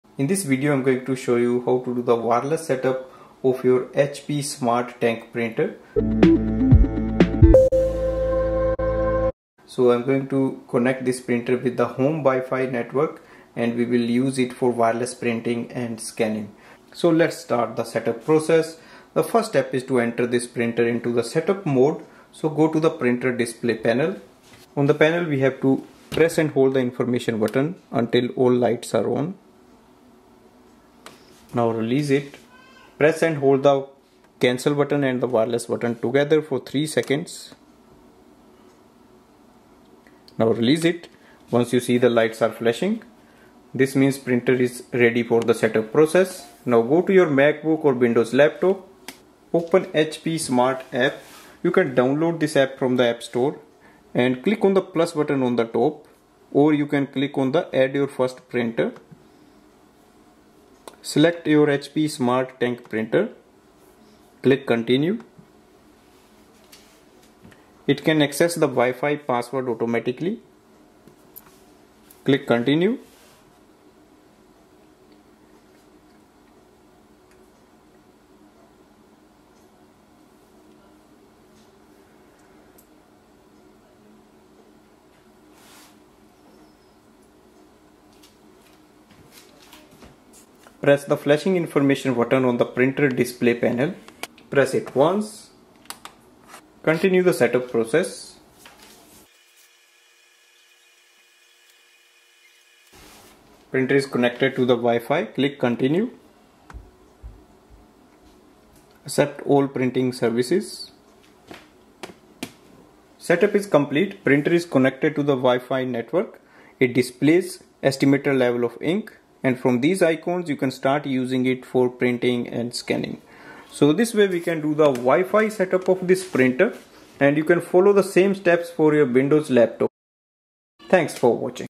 HP Smart Tank 520 | 525 | 540 | 580 | 589 WiFi Setup Using MacBook or Windows | Wireless Setup.
This video reviews how to do the wifi setup of the HP Smart Tank 520, 525, 540, 580, and 589 all-in-one printers using MacBook or Windows Laptop. This includes preparing the HP Smart Printer for wireless setup and then using HP Smart App with Macbook or Windows Laptop.